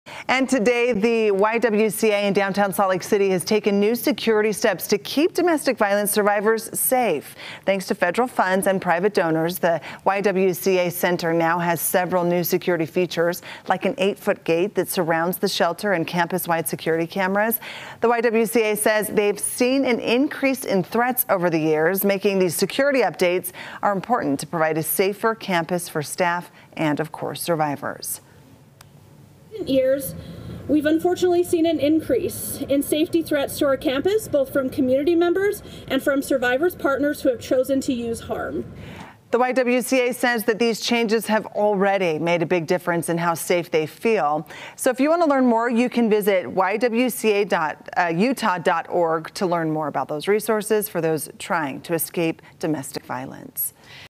YWCA shelter takes new security steps to keep domestic violence survivors safe
New features like an 8-foot gate and security cameras will make the campus safer for everyone.